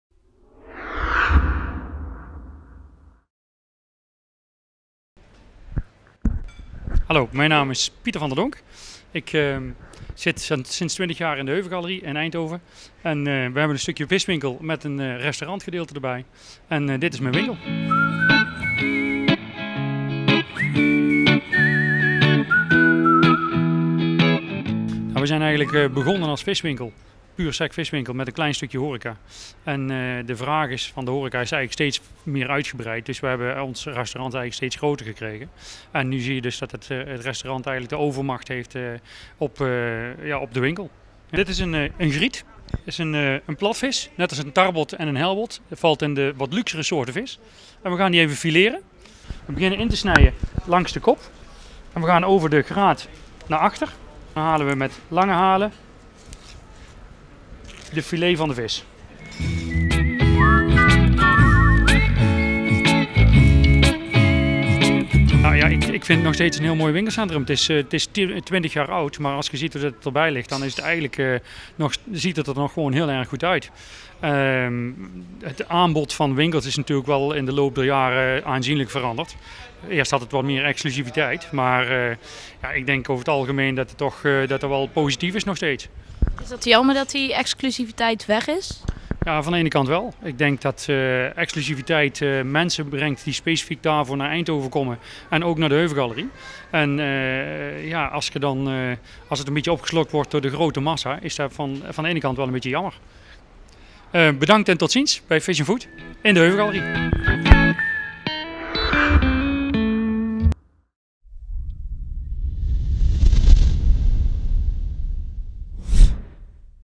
De Heuvel: restaurant neemt overhand viswinkel
De Heuvel siert inmiddels twintig jaar het decor van Eindhoven. Sommige zelfstandige ondernemers zijn al vanaf het begin gevestigd in het winkelcentrum. Hoog tijd om kennis te maken met de winkeliers. ED Nero is benieuwd hoe het is om in zo'n groot complex een winkel te hebben. Is de sfeer nog net zo goed als in het begin?